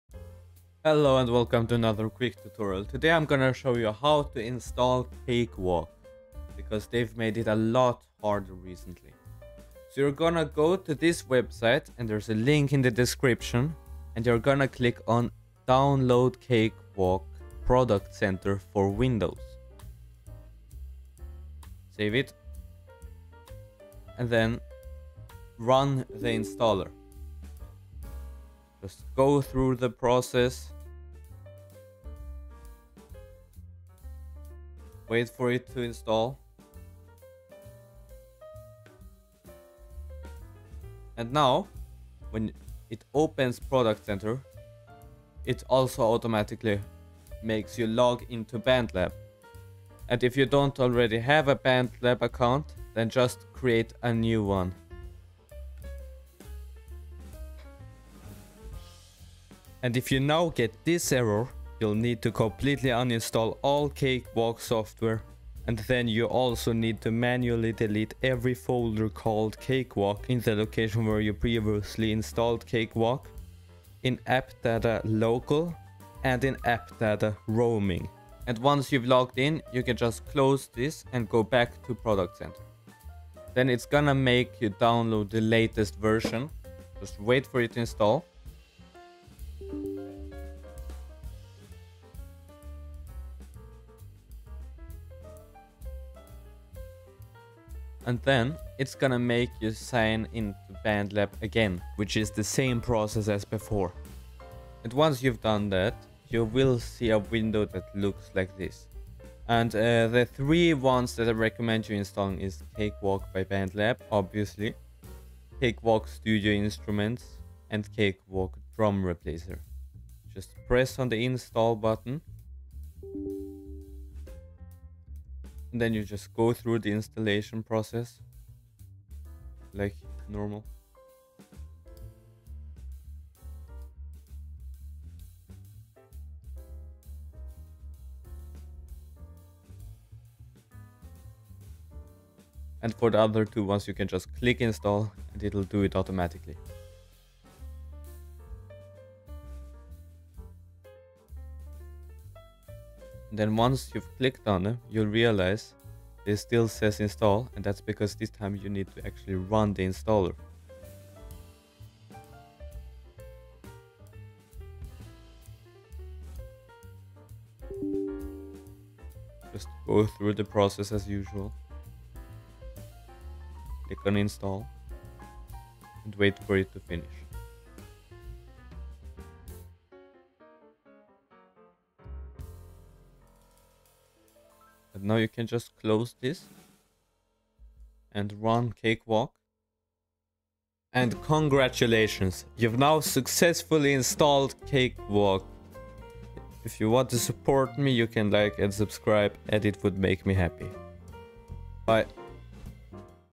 How To Install Cakewalk DAW by BandLab for FREE in 2025 (It still works)
In this video I will show you how to install the amazing and powerful free DAW software Cakewalk by BandLab for free in 2025, because it's not as easy as it used to be. Cakewalk is a very powerful free DAW software for Windows made by BandLab that is great for beginners, but also for more experienced people. Lately they've made it quite a bit harder to download, maybe to make people buy their new softwares Sonar and Next instead. But fear you shall not since here is another Quick Tutorial to show you how to download it.

Unfortunately it seems like this doesn't work for everyone anymore. If this is the case for you, then I would highly recommend another "free" software; Reaper. The reason I say "free" is because it's "nagware", it nags you to pay for it, but you can still use it if you don't. (Just like WinRAR)